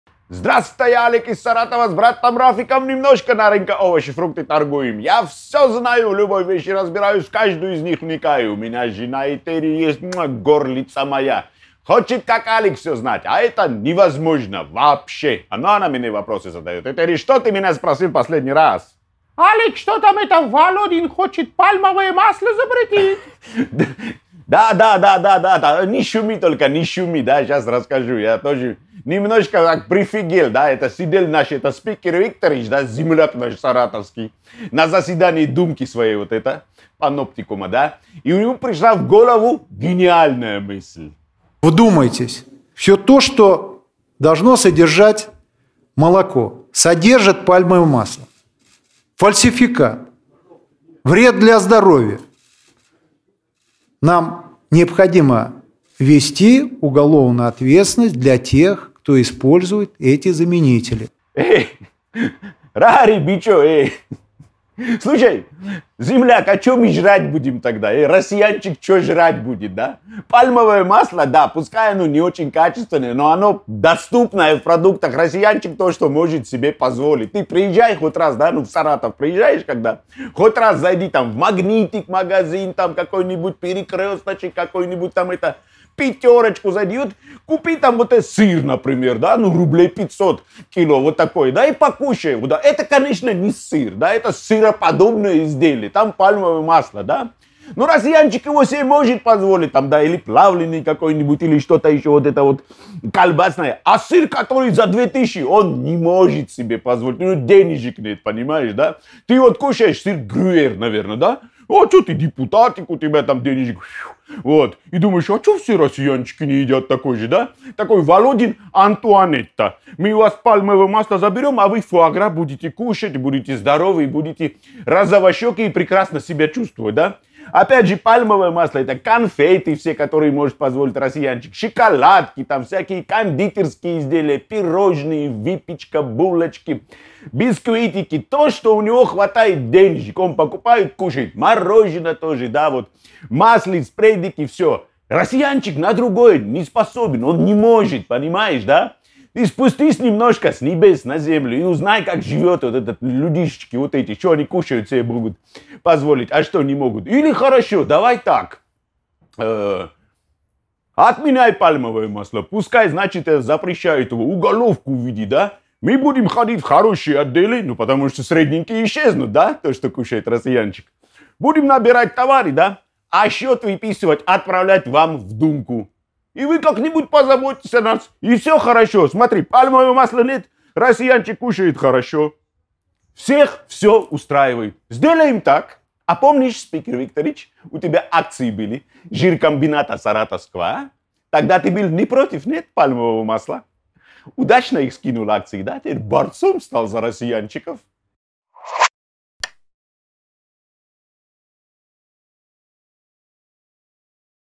Володин против пальмового масла. Алик из Саратова
Спикер Госдумы Вячеслав Володин вдруг предложил ввести уголовную ответственность за использование пальмового масла в пище. И это при том, что когда-то он был акционером жиркомбината и против этого масла ничего не имел.
Но поразительно другое: пальмовое масло, может, и не очень качественный ингредиент, но уж точно не отрава. И это масло входит в состав многих продуктов, которые доступны простому россиянину: сыры, кондитерские изделия, лапша и многое другое.
Алик спрашивает: что тогда будет есть простой россиянин?